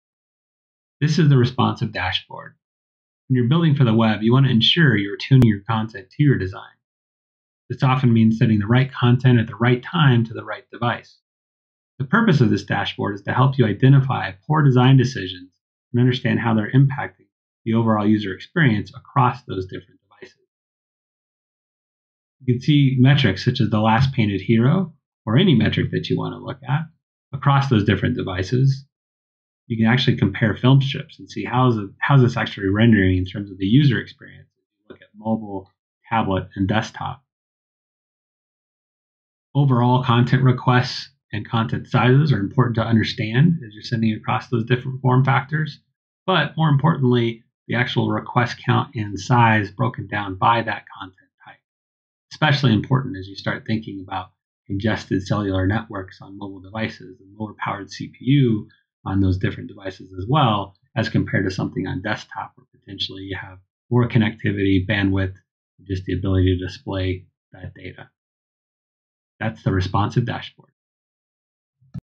SpeedCurve Synthetic – Responsive Dashboard Walkthrough
Understanding how design decisions impact the user's experience of an application across different form factors is key to building a highly performing website. This walkthrough discusses how you can leverage the Responsive dashboard in SpeedCurve to address potential issues brought about in your design.